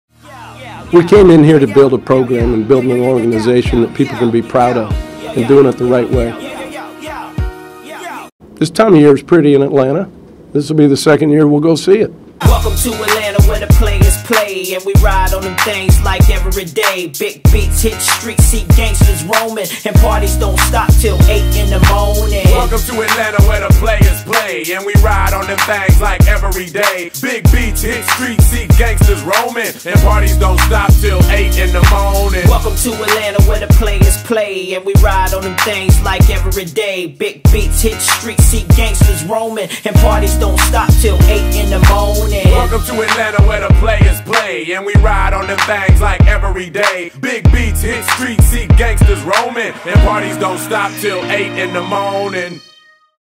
Florida Gators 2016 SEC Championship Hype Up - "Atlanta"
The Florida Gators are your back-to-back SEC East Champions!

Video Clips: Florida Gators YouTube Channel